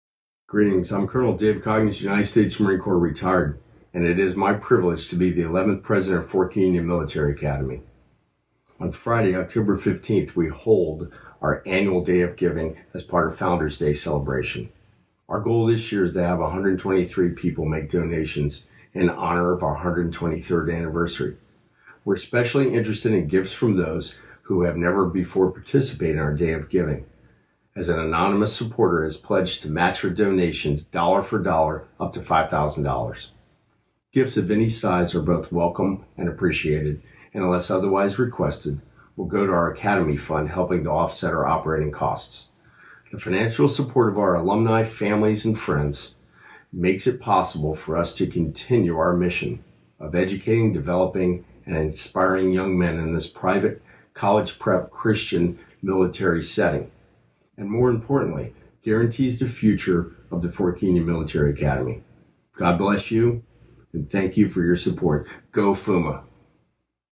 Day of Giving 2021
Colonel Coggins invites all to participate in our annual Day of Giving held on Founder's Day, October 15th, each year. Donations will go to the Academy Fund to support the Academy's operations.